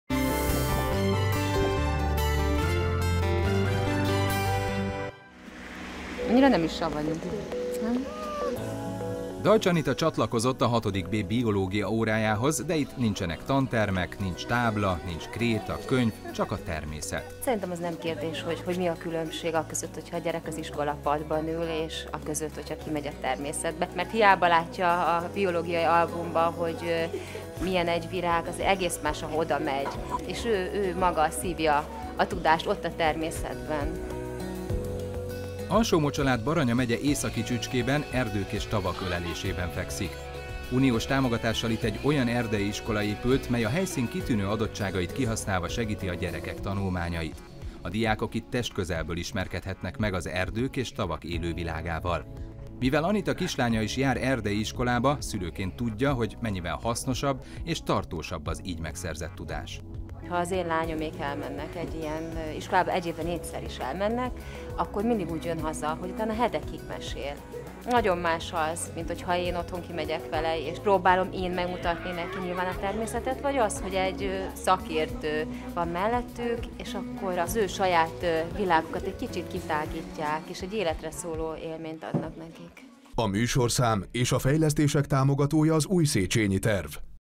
Számít a szavam kampány - Deutsch Anita
Miért állt Deutsch Anita a „Kölyök-Fészek" Erdei Iskola projektje mellé?